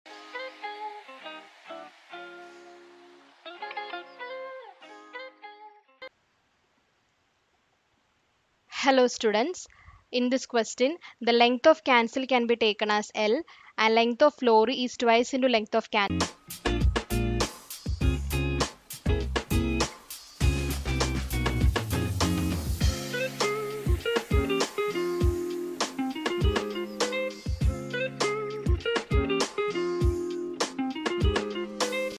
When measured at rest, Super Lori is twice as long as a Perodua Kancil. As the two vehicles were go…
When measured at rest, Super Lori is twice as long as a Perodua Kancil. As the two vehicles were going through a speed trap, Super Lori overtakes the Perodua Kancil. At the moment of overtaking, the policeman observes that the length of both vehicles is the same. The Perodua Kancil was moving at half the speed of light. How fast was Super Lori moving?